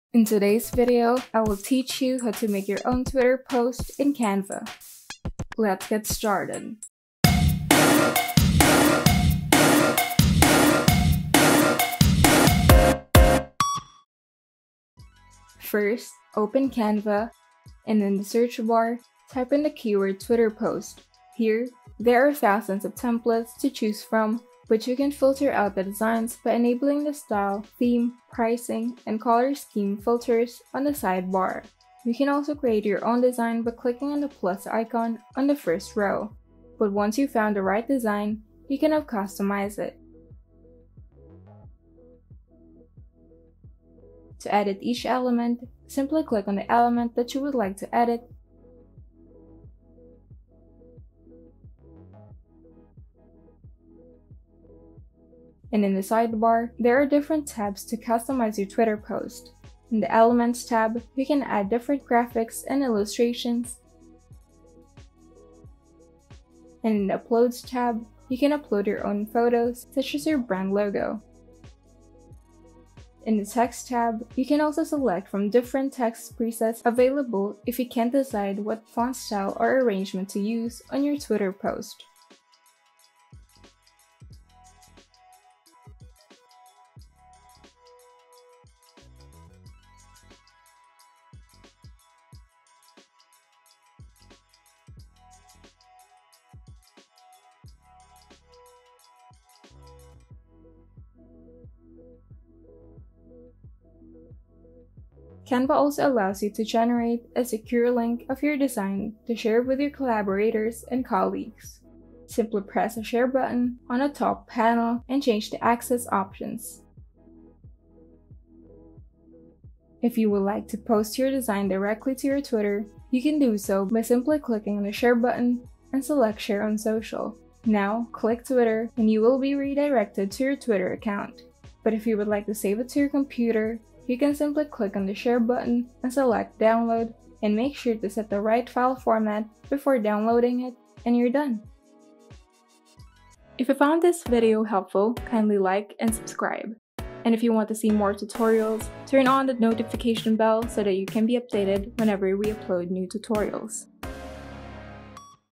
How to Create Awesome TWITTER POSTS Using Canva (FREE Templates)
Images can be a powerful tool to connect with your Twitter followers.  You can create amazing and professional graphics for your Twitter posts in a matter of minutes using Canva.  Watch this tutorial to see how to get started!

0:00 (Intro) Create Awesome Twitter Posts in Canva
0:15 Selecting Your Template
0:43 Customizing Elements
1:06 Uploading Your Own Images & Text
1:45 Collaborating
1:59 Posting Directly to Twitter
2:11 Saving Your Twitter Image